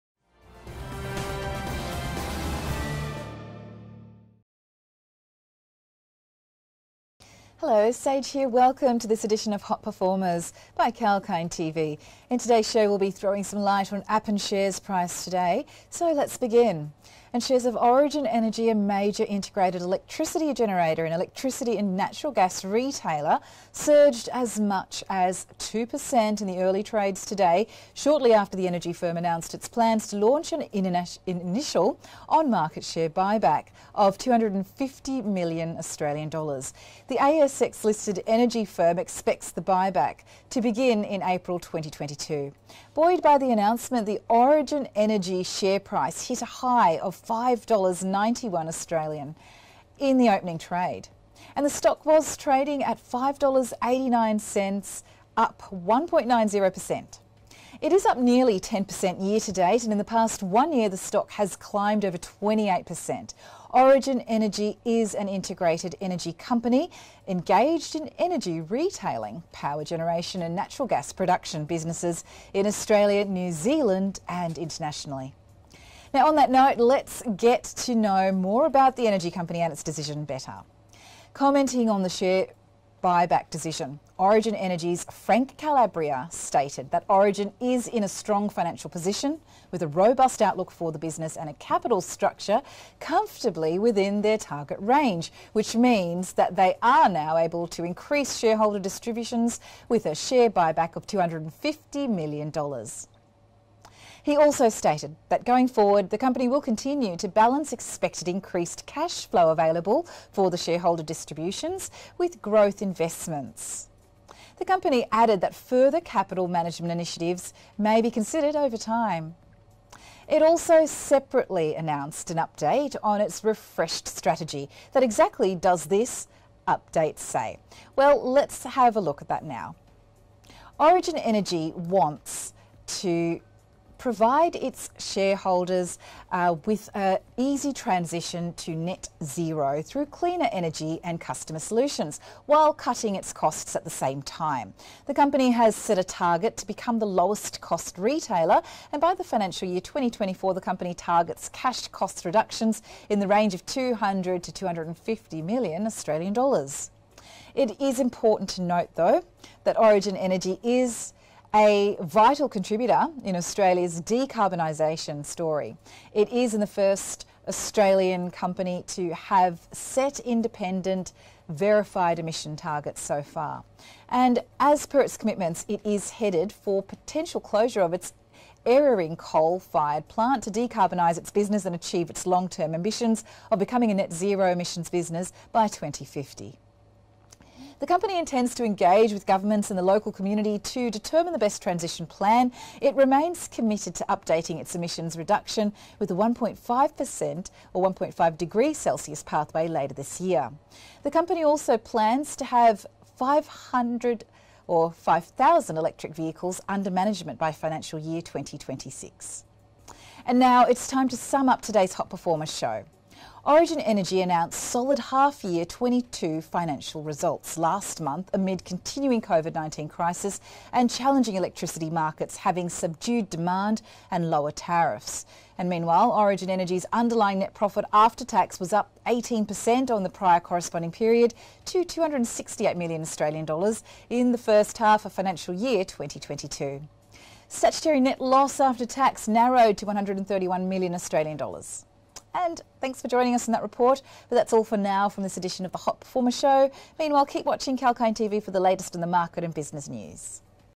How Are Origin Energy (ASX:ORG) Shares Reacting to AU$250M Buyback?
Origin Energy has announced its plans to undertake an initial on-market share buyback of AU$250 million. The ASX-listed energy firm expects the buyback to begin in April 2022. In a separate announcement, the company shared its plans to have 5,000 electric vehicles under management by FY 2026.